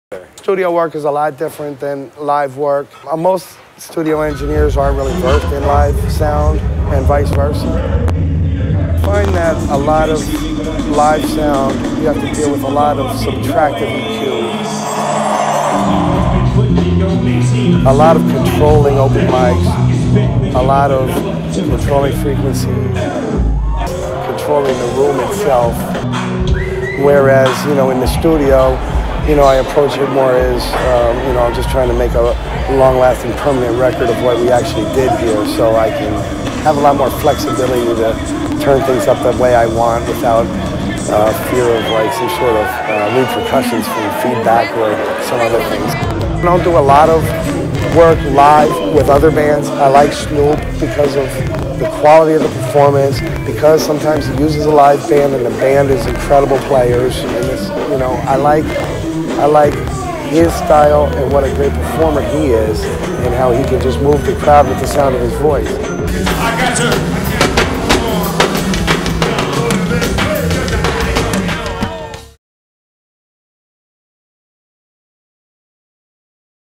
R. I. P. Dave Aron
R.I.P. Dave Aron (Sound Engineer)

@davedizzle420 was really cool and showed us a few of his personal @wavesaudio plugins that he used for @snoopdogg vocals and gave us an XLR plug in into the mixing board that he controlled with his laptop.